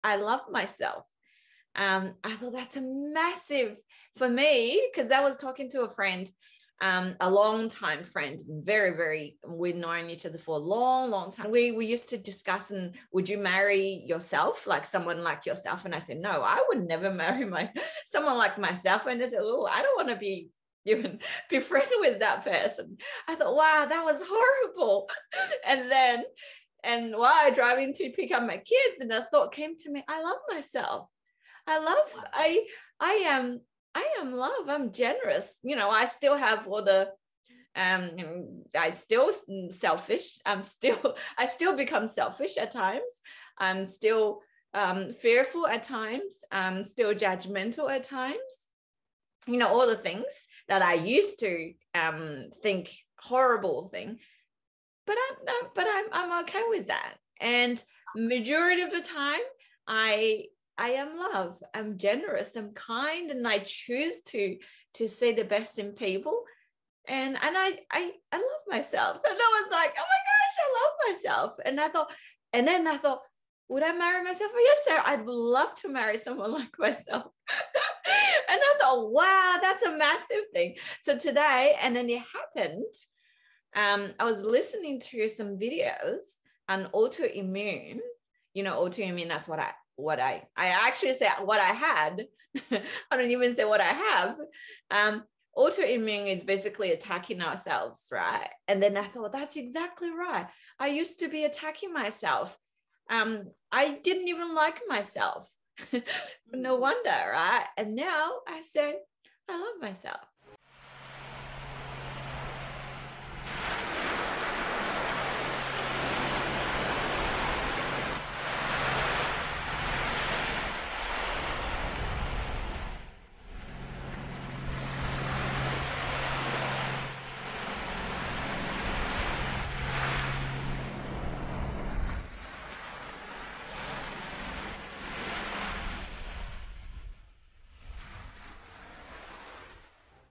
I Love Myself - Let Your Light Shine Podcast
Would you marry yourself? A few years ago, Annie Deng would say, "no, of course not"!

Today Annie says “I love you and I love myself too”! After all, you can't give what you don't have.

If you don't even love yourself, how do you expect anyone to love you, or how do you go about loving others?

Let your light shine, and help others find their way out of the darkness!